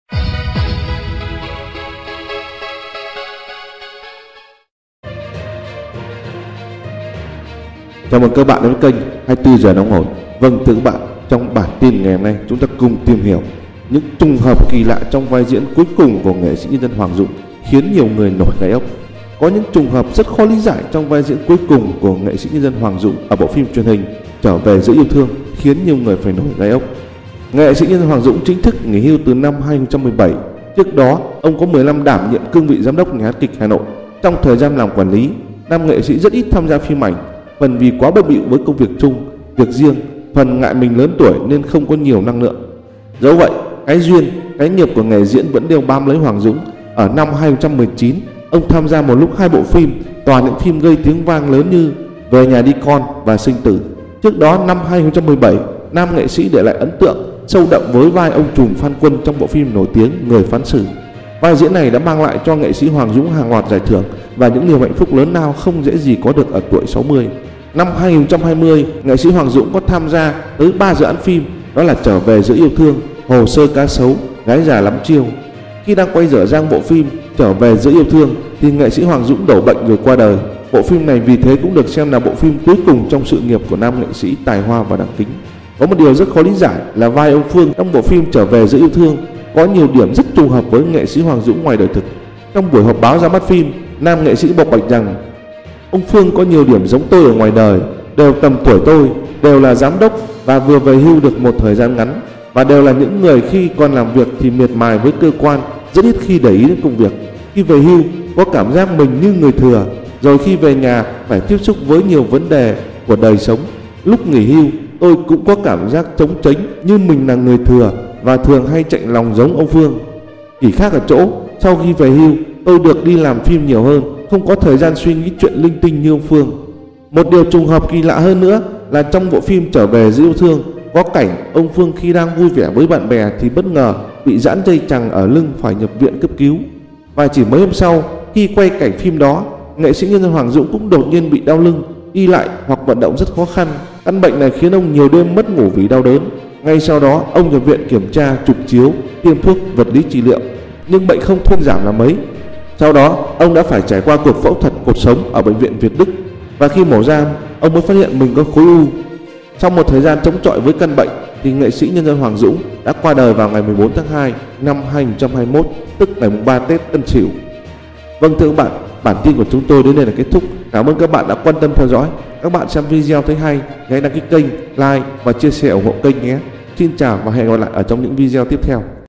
Cố NSND Hoàng Dũng được ‘‘Điểm gở’’ báo trước cái chết
Một điều trùng hợp kỳ lạ hơn đó là trong phim "Trở về giữa yêu thương" có cảnh, ông Phương khi đang vui vẻ với bạn bè thì bất ngờ bị giãn dây chằng ở lưng phải nhập viện cấp cứu. Và chỉ mấy hôm sau khi quay cảnh phim đó, NSND Hoàng Dũng cũng đột nhiên bị đau lưng, đi lại hoặc vận động rất khó khăn. Căn bệnh này khiến ông nhiều đêm mất ngủ vì đau đớn. Ngay sau đó, ông nhập viện kiểm tra, chụp chiếu, tiêm thuốc, vật lý trị liệu... nhưng bệnh không thuyên giảm là mấy. Ông phải trải qua một cuộc phẫu thuật cột sống ở Bệnh viện Việt Đức (Hà Nội).